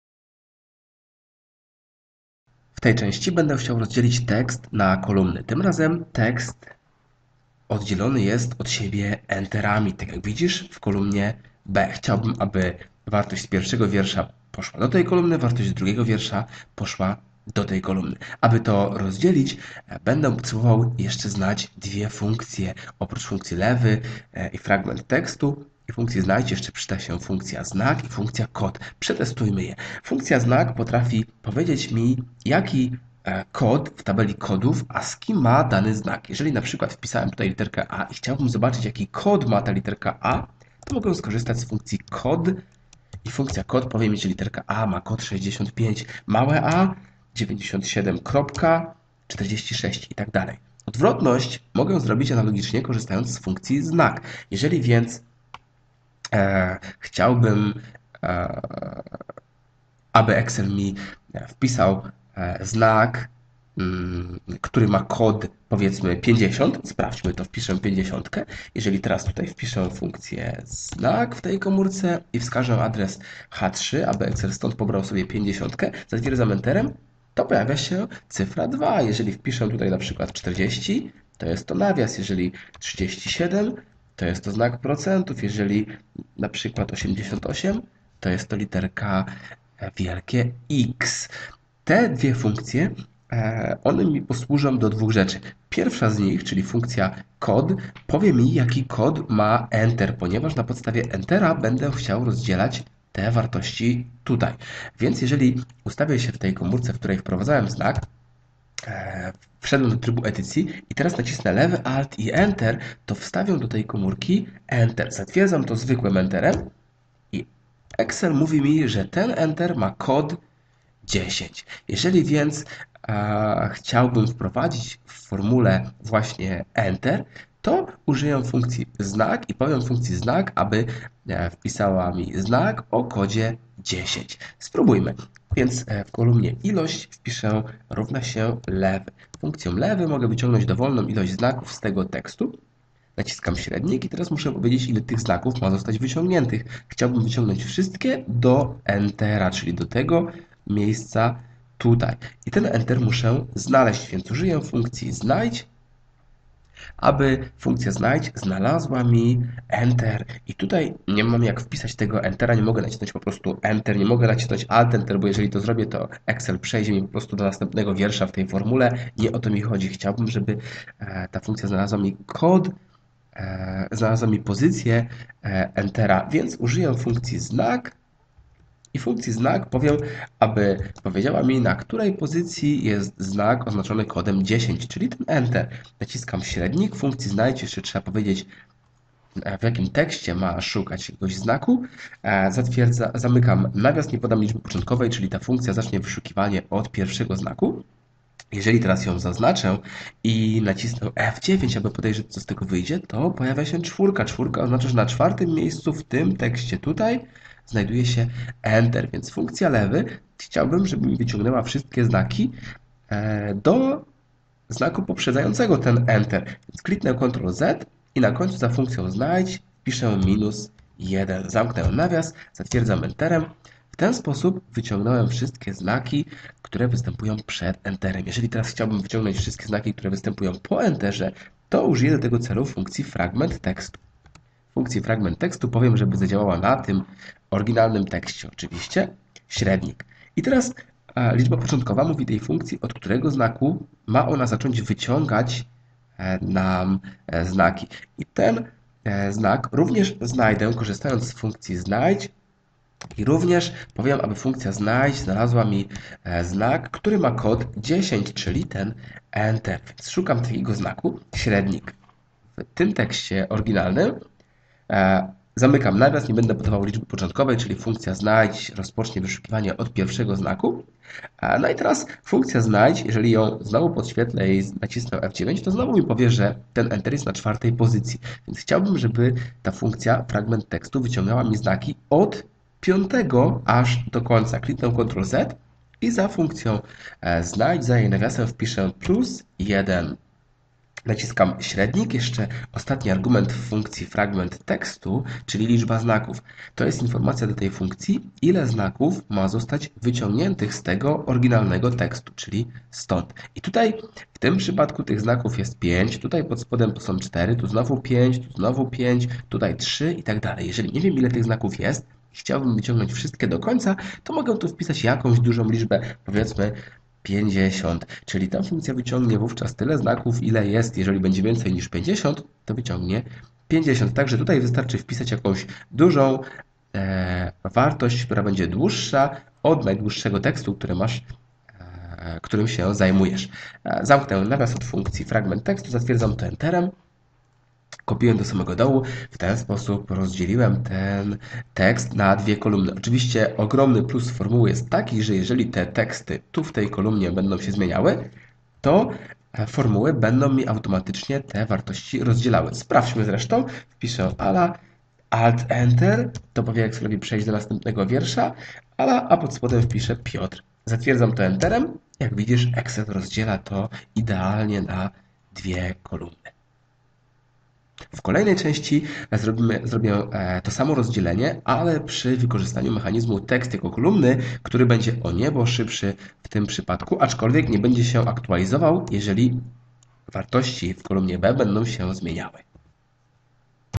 excel-133 - Rozdzielenie tekstu z 'enterami' (LEWY, ZNAJDŹ, ZNAK, FRAGMENT.TEKSTU, KOD)
W tym odcinku możesz zobaczyć jak rozdzielać tekst znajdujący się w kilku wierszach jednej komórki do osobnych kolumn przy pomocy formuły.
Formuła ma ten plus, że aktualizuje się, jeśli zmienią się również dane źródłowe.
Użyta/e funkcja/funkcje LEWY, ZNAJDŹ, ZNAK, FRAGMENT.TEKSTU, KOD